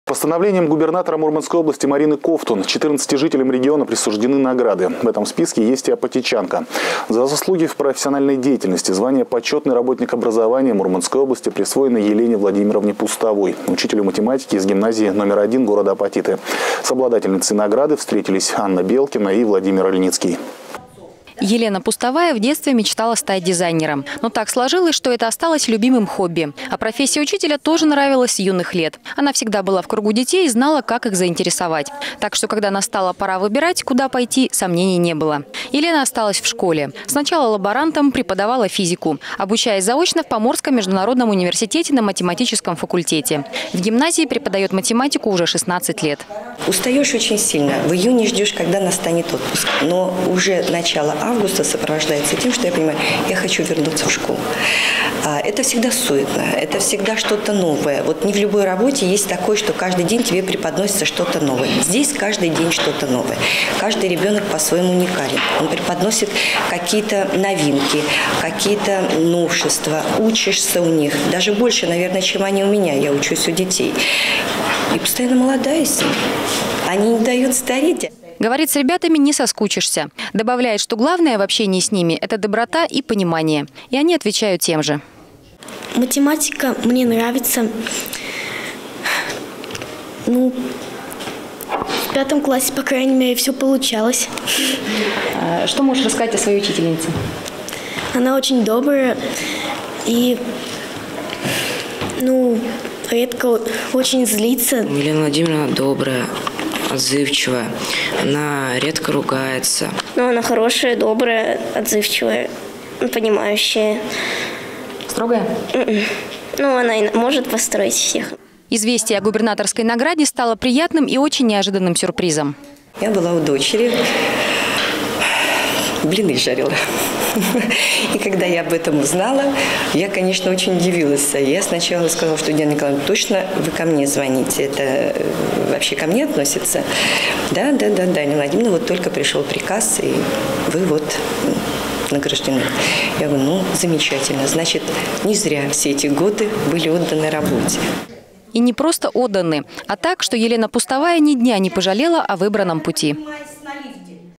14-ти жителям региона присуждены награды
Постановлением губернатора Мурманской области Марины Ковтун 14-ти жителям региона присуждены награды. В этом списке есть апатитчанка. За заслуги в профессиональной деятельности звание «Почетный работник образования Мурманской области» присвоено Елене Владимировне Пустовой, учителю математики гимназии № 1 города Апатиты. С обладательницей награды встретились Анна Белкина и Владимир Ильницкий.